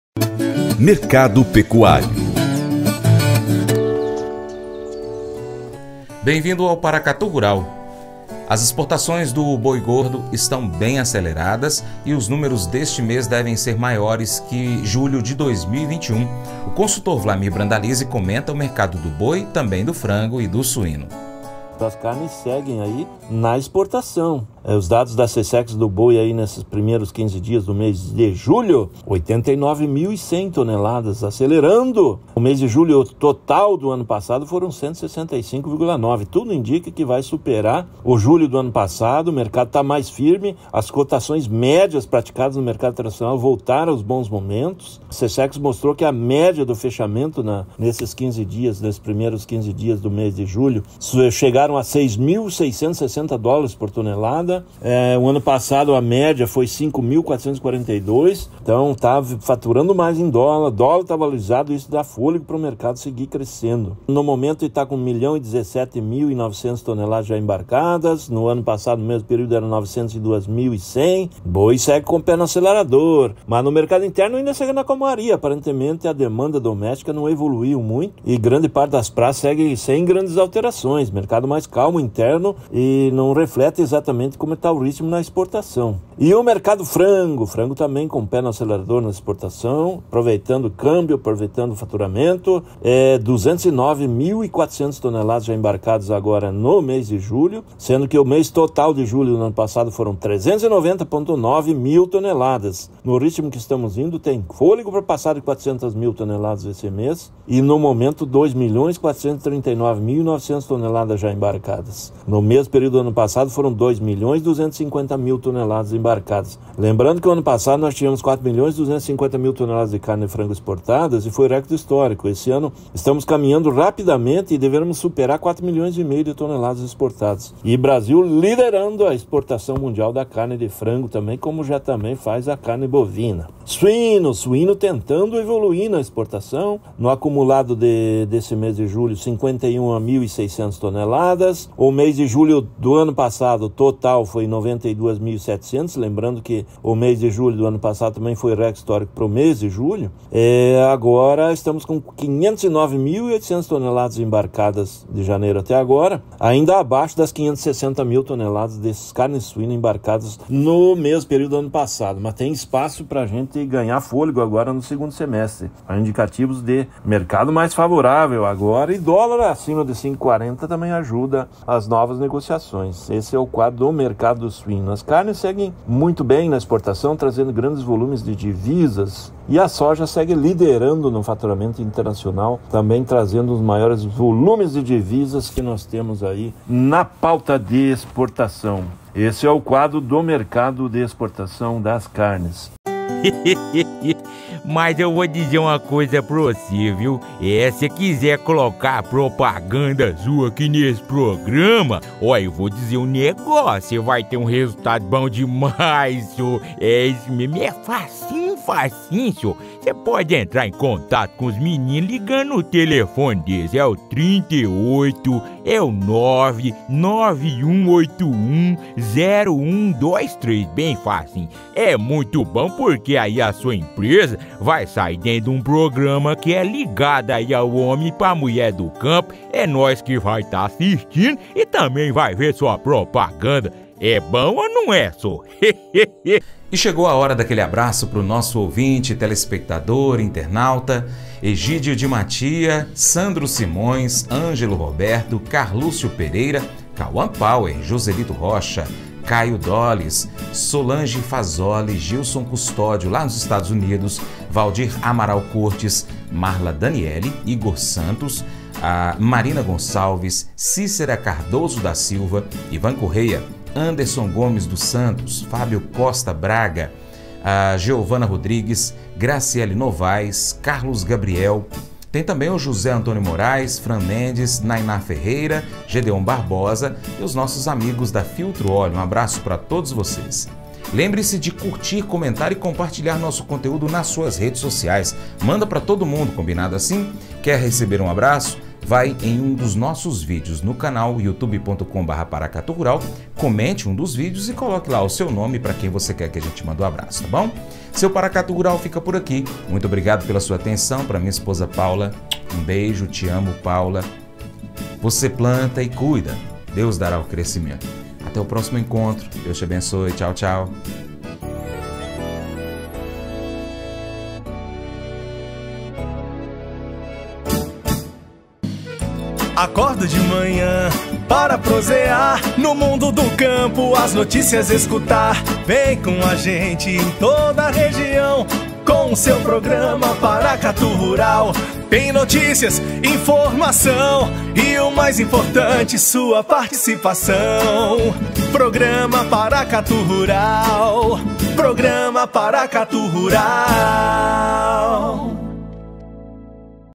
Mercado do BOI, FRANGO e SUÍNO 20-07-2022
Exportações do BOI GORDO estão bem aceleradas, e números deste mês devem ser maiores que julho de 2021. Vlamir Brandalizze comenta o mercado do BOI, do FRANGO, e do SUÍNO.